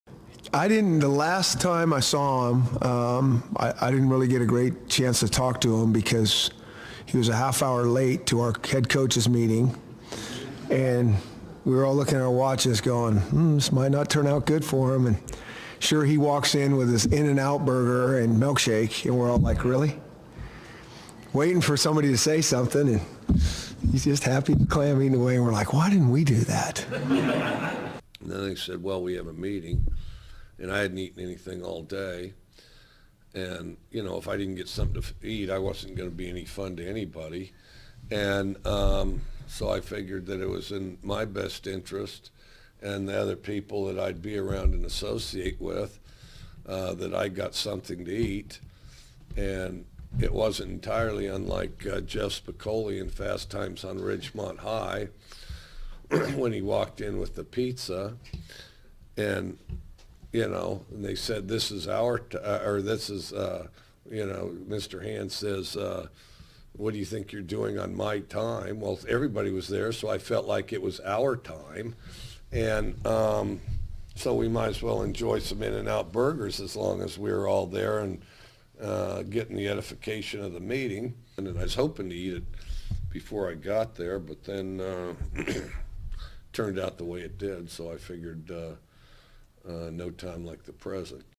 Last Time for Petersen and Leach Saw Each Other 2018-11-20
Washington Coach Chris Petersen says the last time he saw Washington State Coach Mike Leach was at a Pac-12 Coaches meeting.  Leach was late and walked in eating Inn-N-Out.  The Cougars and Huskies face off in the 110th Apple Cup Friday in Pullman with a kickoff at 5:30 on FOX-TV.